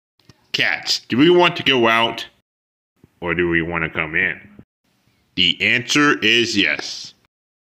Garfield Comic Dub for June 29th, 2023
Garfield shows you if cats want to go in or come out.

Garfield is owned by Jim Davis, PAWS Incorporated, Nickelodeon and Paramount Global.